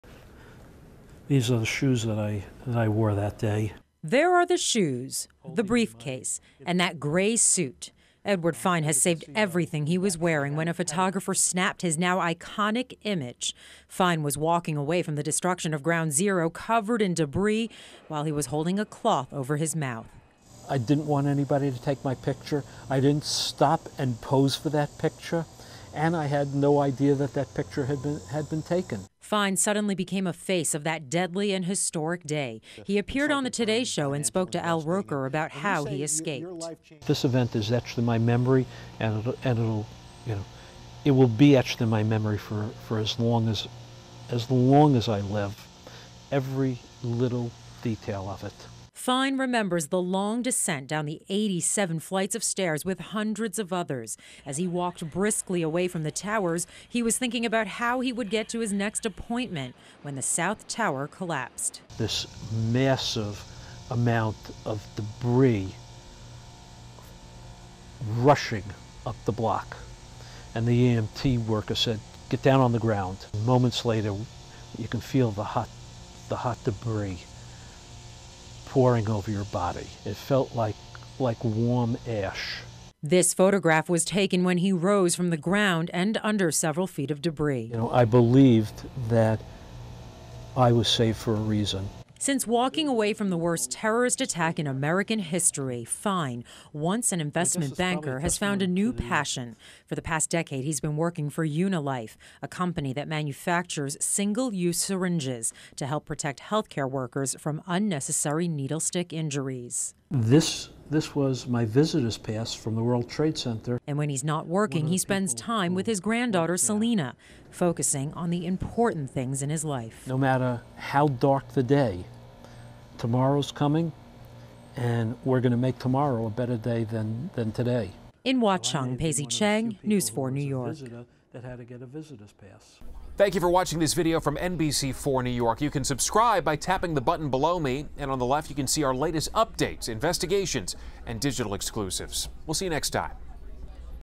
9/11 Anniversary: Man in Famed Dust Photo Recounts WTC Attacks | NBC New York Archives
The black leather Cole Haan shoes, the leather briefcase, and the gray suit are like relics in a museum in Edward Fine’s closet.

“These are the shoes I wore that day," he says. "The suit is the same, just clean of course.”

Fine has saved everything he was wearing on Sept. 11, 2001, when a photographer snapped a photo of him walking away from the twin towers, covered in dust and debris, holding a cloth over his mouth. The photograph later became an iconic image from the day of the attacks.